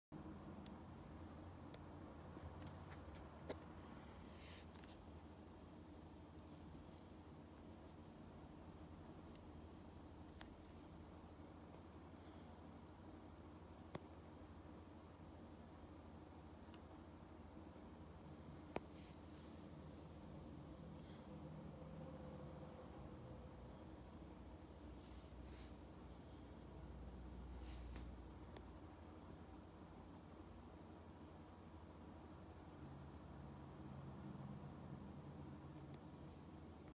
Messor Barbarus: Feeding them some honey
They seemed to need some sugar to get some energy so I thought they need some honey.